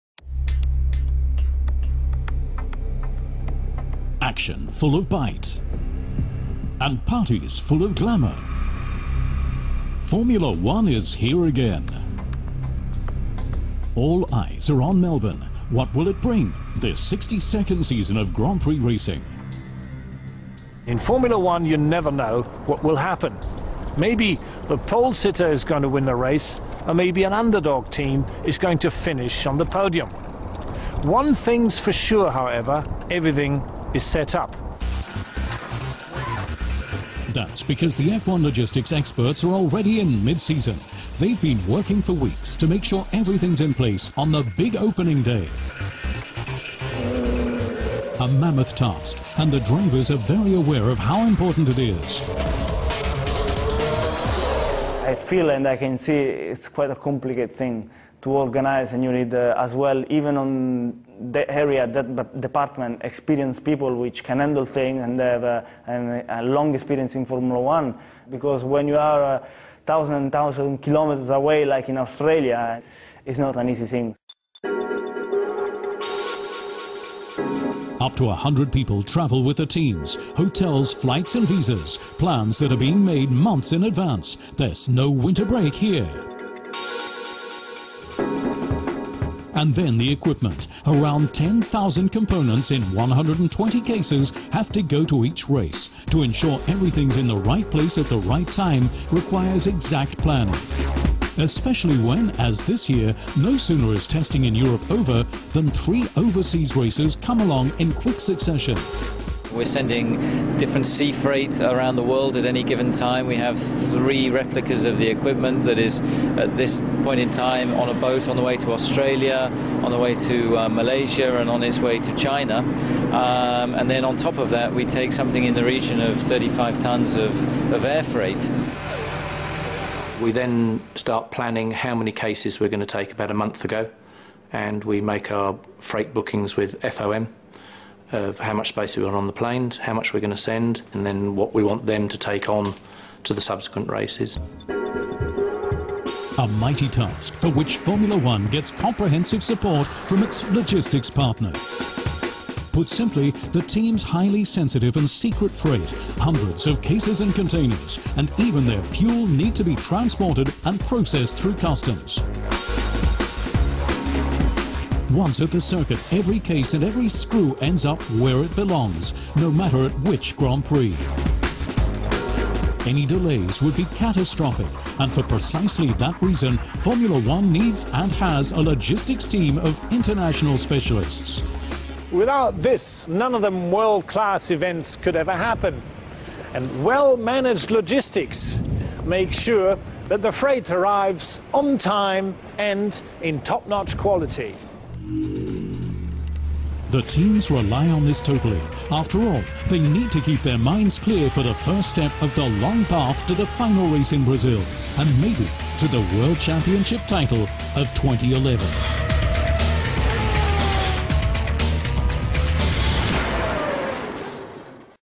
La logística de la Fórmula 1
Mover todo el equipo de Fórmula 1 es una tarea titánica y aquí te mostramos cómo la firma de mensajería y paquetería DHL se encarga de movilizar a la máxima categoría por todo el mundo